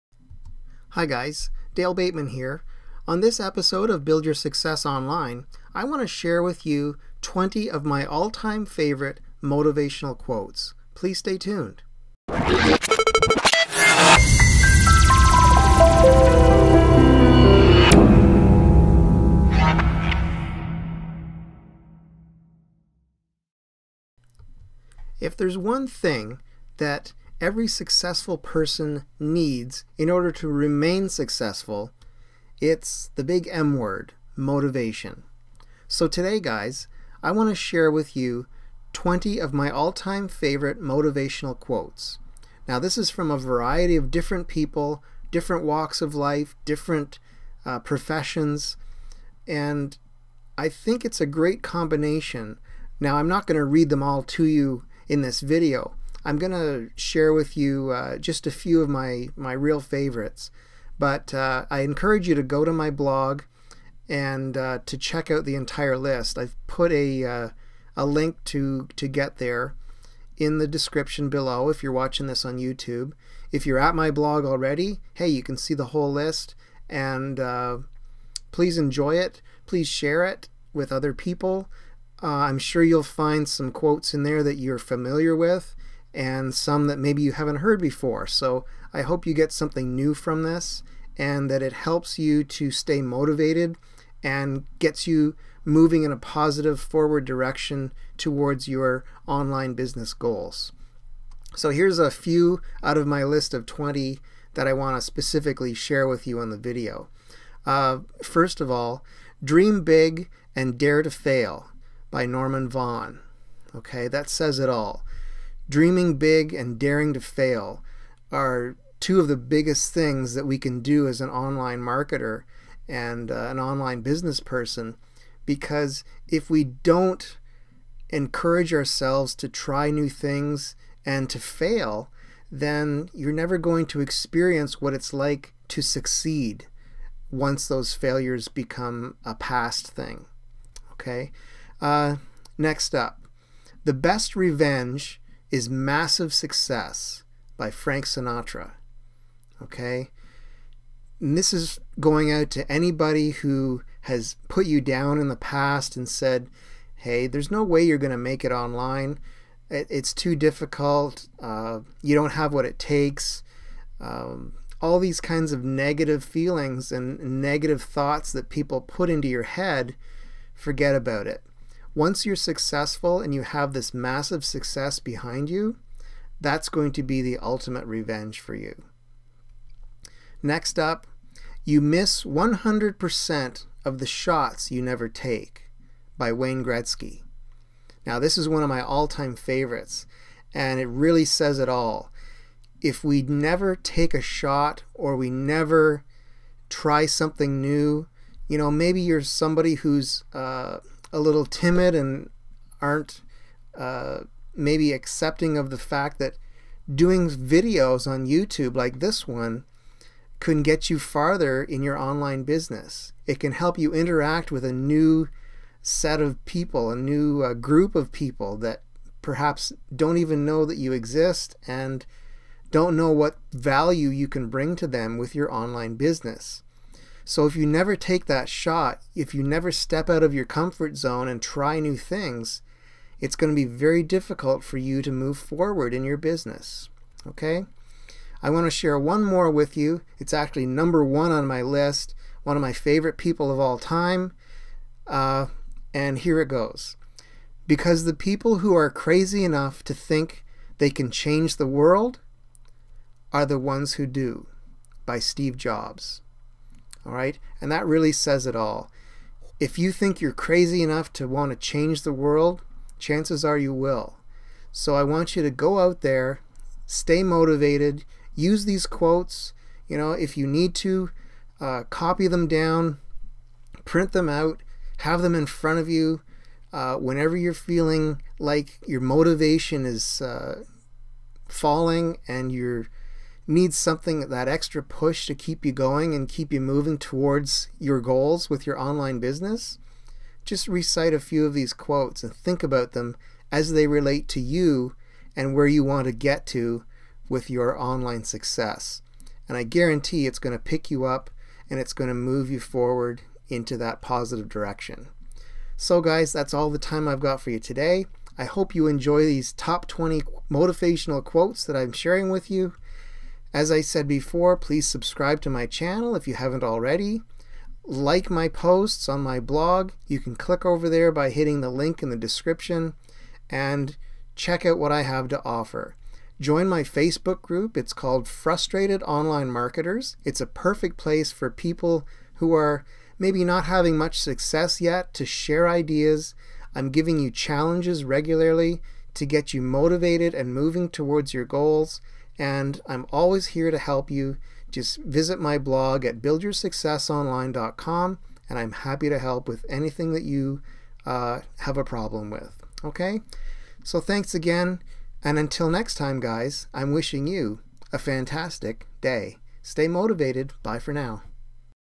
Top 20 Motivational Quotes of All Time
One of the things that every successful person in business, sports, entertainment or whatever they do for a living, has in common is a steady stream of motivation fueling their desire for success.

I often tell folks that in order to become and remain successful, you must have the motivation to keep going, never giving up on your dreams. In order to help you stay positive, and to move you forward on your own road to success, I want to share with you my top 20 motivational quotes.

I have carefully chosen each of these in order to give you the drive and the desire that you need in order to overcome that next hurdle that's always waiting for you, and to push you to do things that, may seem more than a little uncomfortable at first, but, when you allow these things to happen, and also to blossom into becoming a regular part of your life and your business, your personal and professional growth will only be matched by your will to continue to succeed.

I hope that you will enjoy these top 20 motivational quotes, and not just read them once and forget about them, but copy them, print them out and paste them in front of you wherever you are working so that you can look at them over and over.

Here's a few of my top 20 Motivational Quotes of all Time:

18. Dream big and dare to fail. --Norman Vaughan

14. The best revenge is massive success. --Frank Sinatra

9. You miss 100% of the shots you don't take. --Wayne Gretzky

1. Because the people who are crazy enough to think they can change the world are the ones who do. --Steve Jobs

Whatever the circumstance, I'm sure that you will agree that having a steady stream of motivation and inspiration like what's represented here is a real propellant for you and also for your online business, making you an almost unstoppable force that is bound for success.

I hope you find these top 20 motivational quotes helpful for building YOUR success online!

I appreciate you, and I wish you all the best for your success!

Build Your Success Online Today!

Cheers,